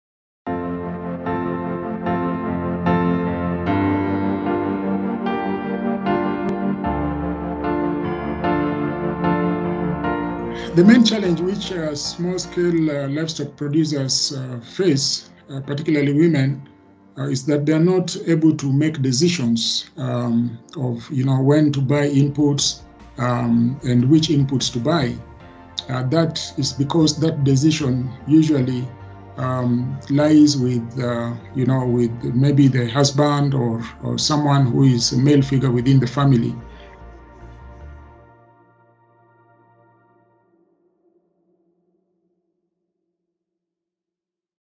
The challenges facing women small-scale producers - Johnson Ouma interview
Johnson Ouma, Executive Director of R&D at GALVmed, addresses one of the challenges faced by women small-scale producers in low-and middle-income countries.